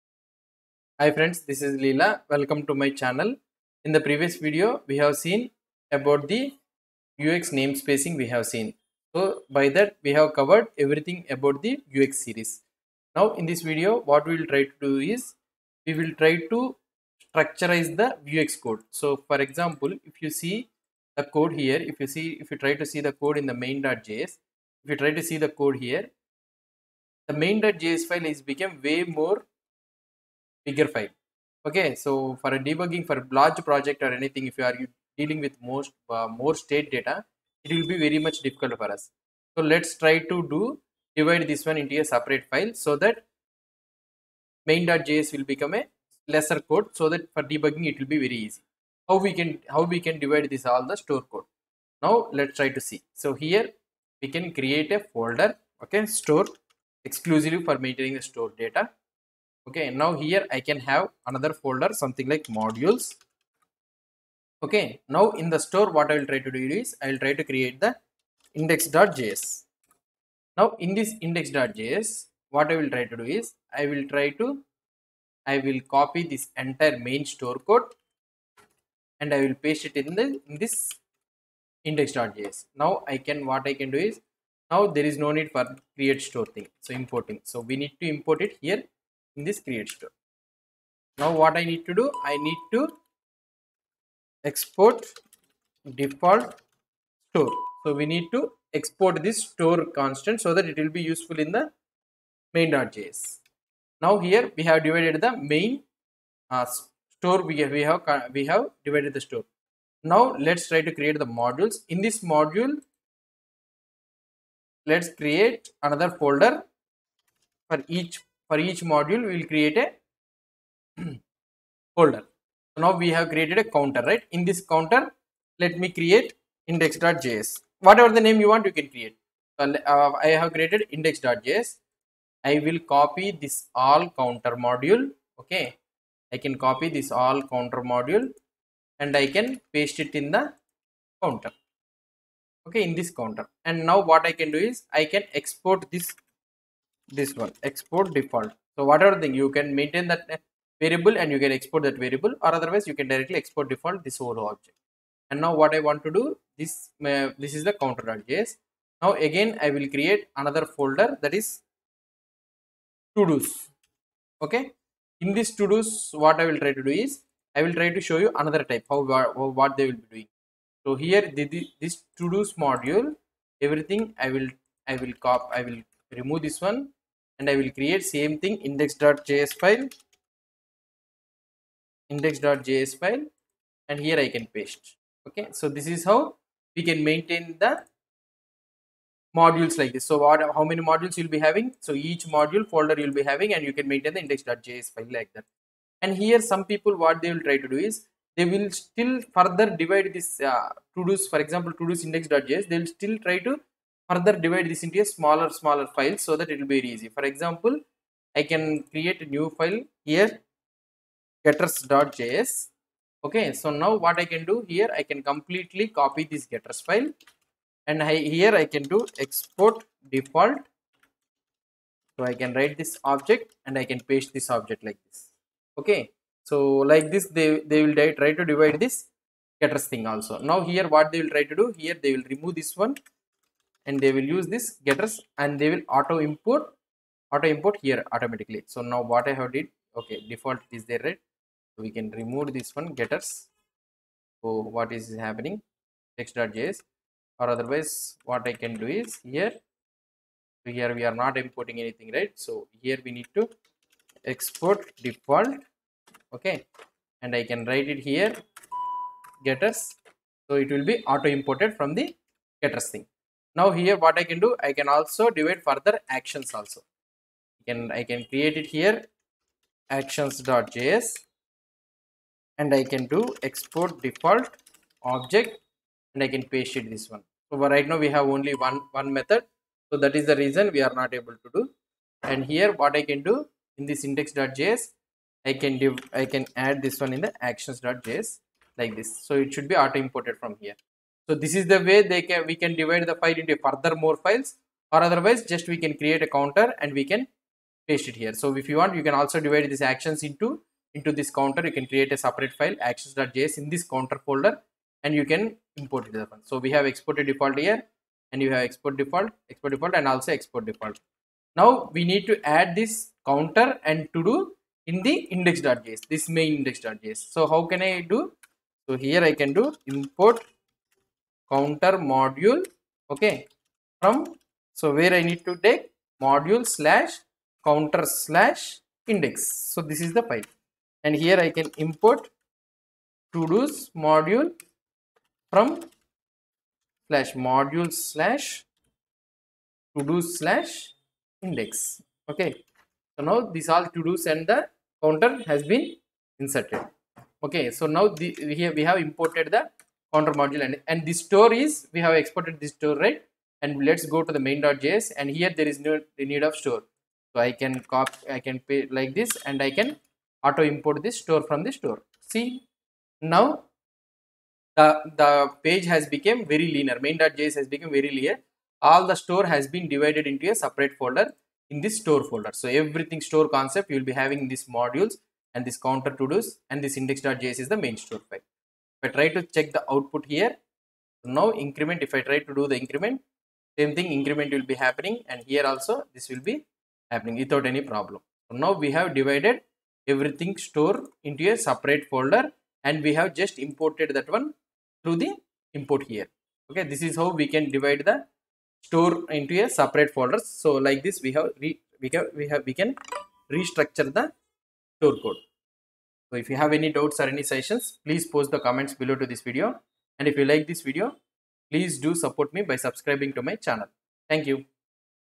82. Managing the Vuex store code in different files and import to single file - Vue js | Vue 3.
Hi Friends
In this video, we will see how to import the store code present in the different files in Vue js | Vue 3.